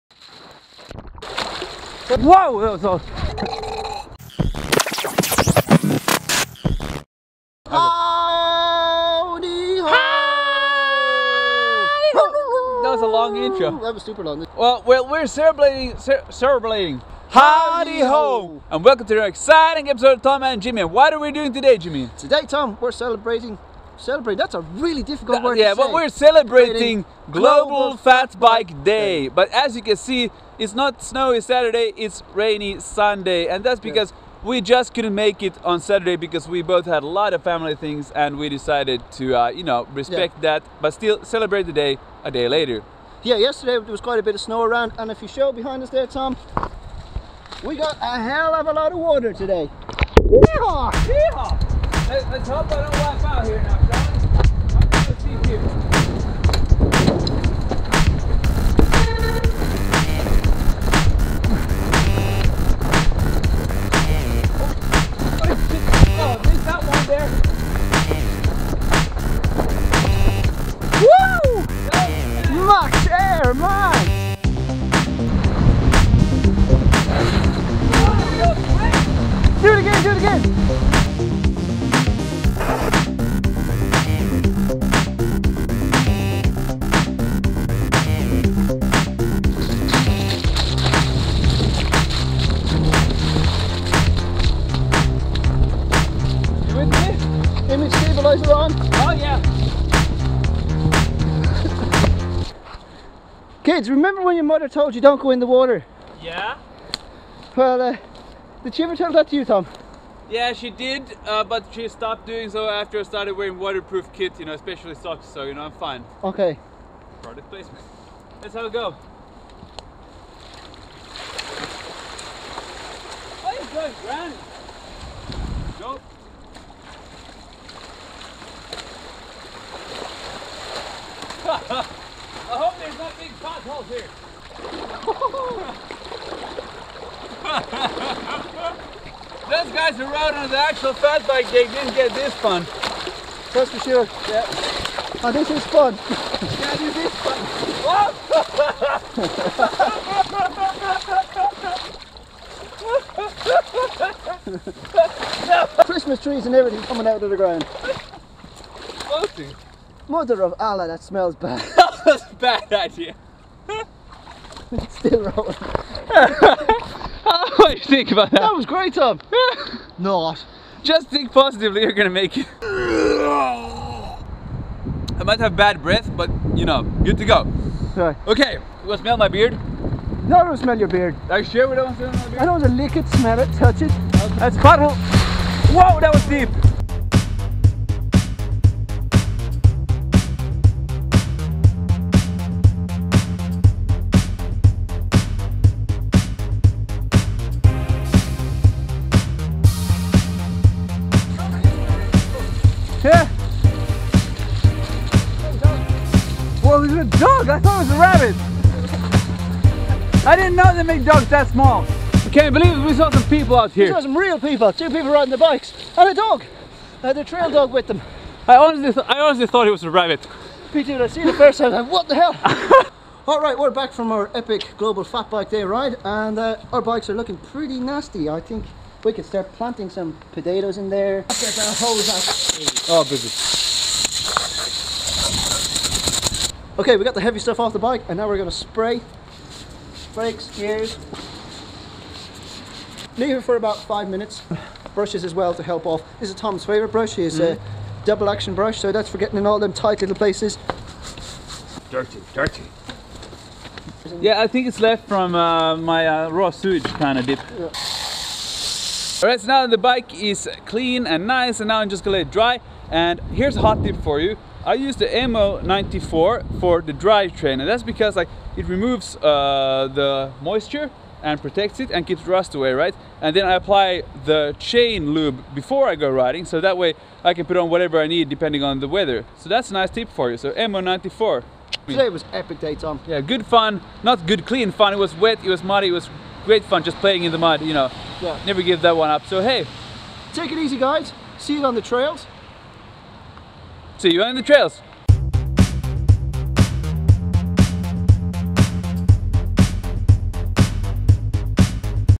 GLOBAL FATBIKE DAY 2017
To be honest, we missed the actual day (December 2nd) because of familystuff so we rode a day later. Saturday was snowy and nice, sunday was the rainiest and darkest day in a while, but who cares, we had a great time riding our fatbikes